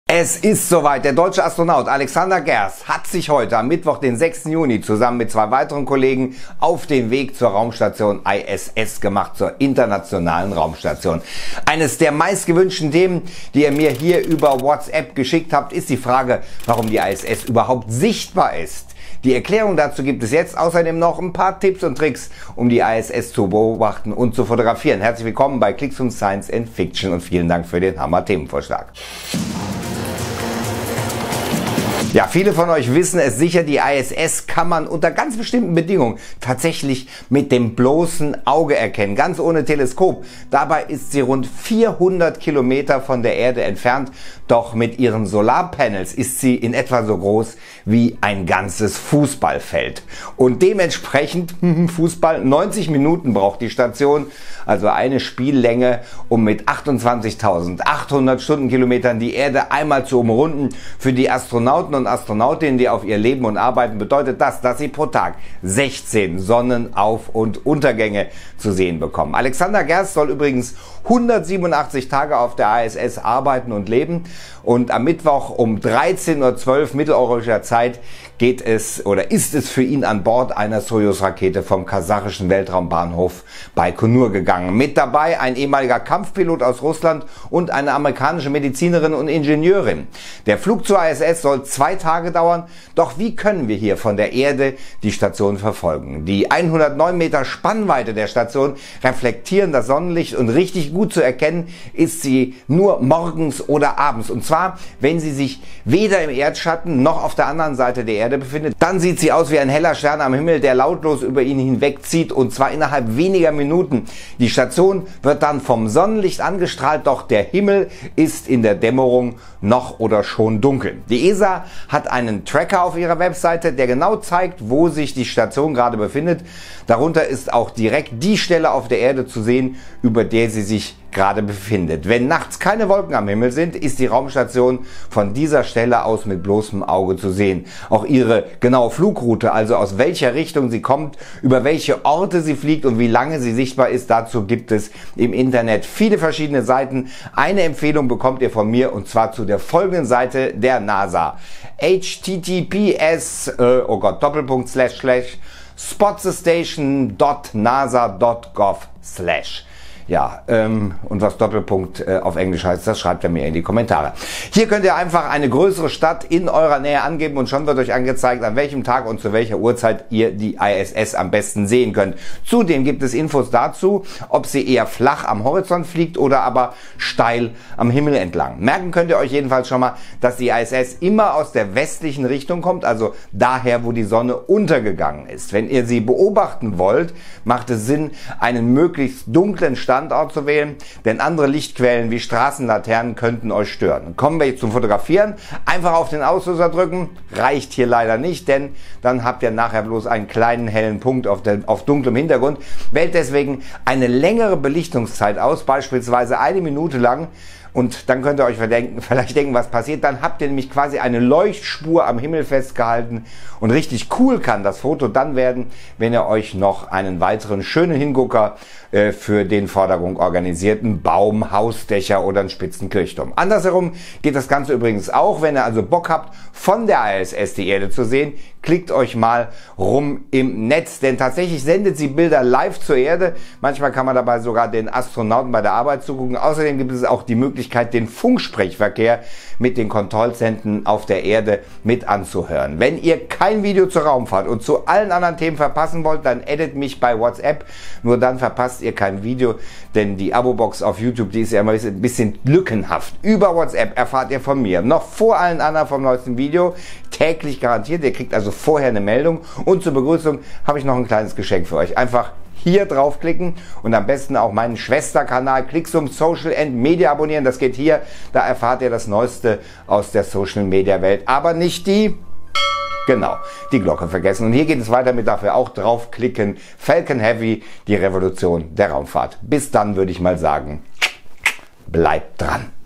Deutscher Astronaut Alexander Gerst auf dem Weg zur ISS - Warum man die ISS tatsächlich sehen kann!
Der deutsche Astronaut Alexander Gerst wird sich heute am Mittwoch, den 6. Juni zusammen mit zwei weiteren Kollegen auf den Weg zur Raumstation machen.

Quelle:

Clixoom Science & Fiction: Das sind spannende News aus der Wissenschaft über revolutionäre Forschungsergebnisse, spannende Studien und spektakuläre Eindeckungen.